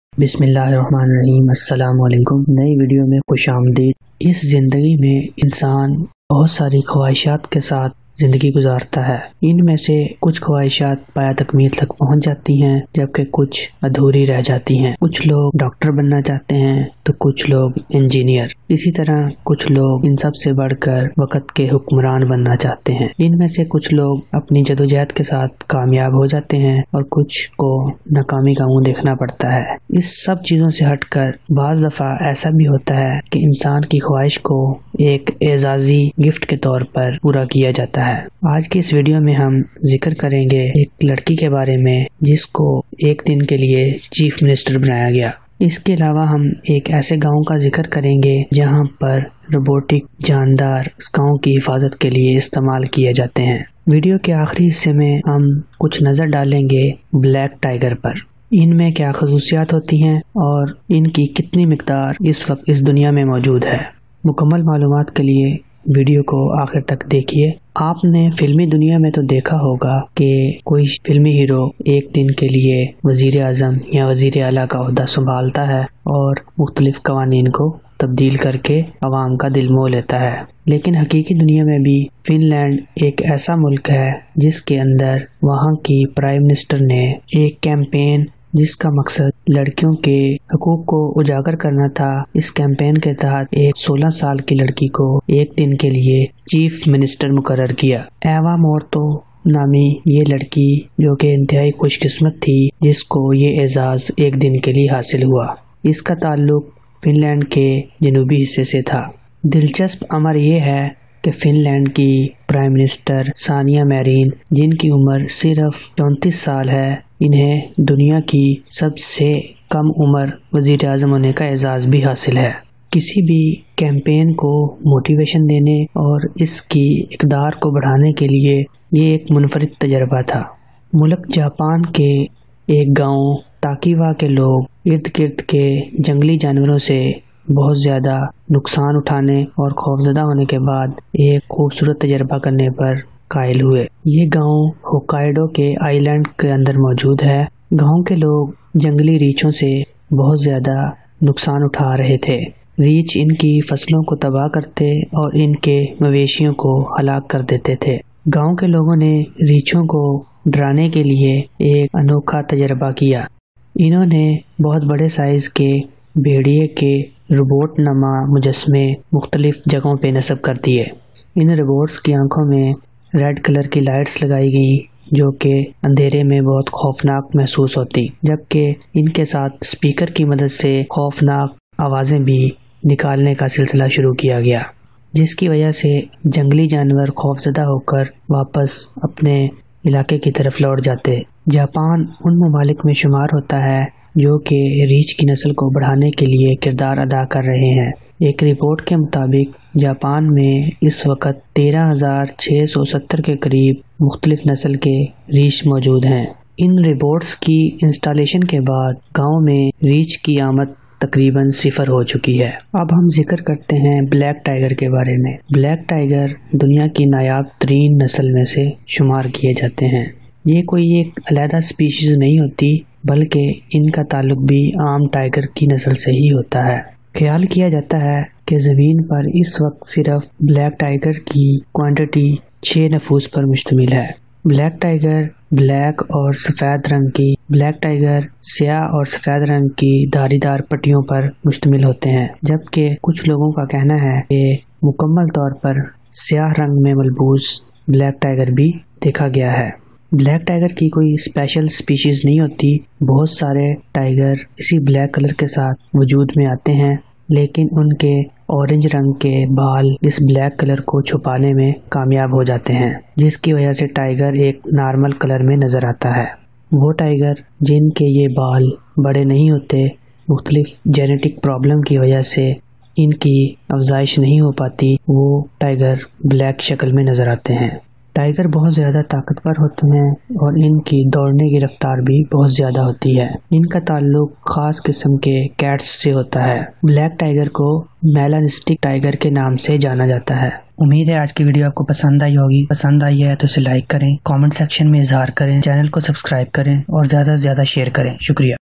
Top |interesting| information, Black |tiger| 1-day |chief minister| robotic |wolf| youngest |pm
Top |interesting| information, Black |tiger| 1-day |chief minister| robotic |wolf| youngest |prime minister|
Wishes of human,
Who was the Chief minister for 1 day,
Robotic guard for village,
About black tiger,
How much quantity of black tiger in present time,
Which country make 1-day chief minister,
Youngest prime minister of world,
Why people experiment of robotic wolf,
What was the success result of robotic guard?,
How much quantity of bears in japan,
How black tigers color turn black,
Tigers belong to which species,